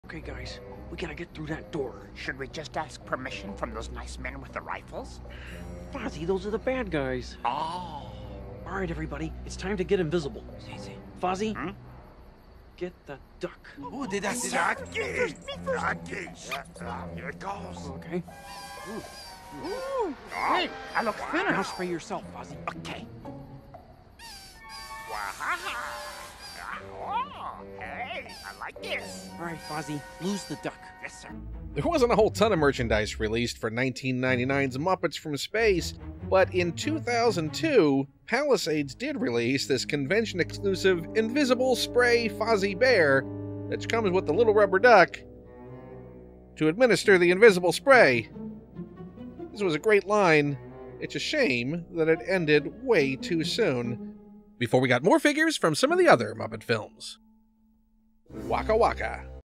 Toy Spotlight - Muppets From Space Invisible Spray Fozzie
A spotlight on the Muppets From Space Invisible Spray Fozzie Bear figure…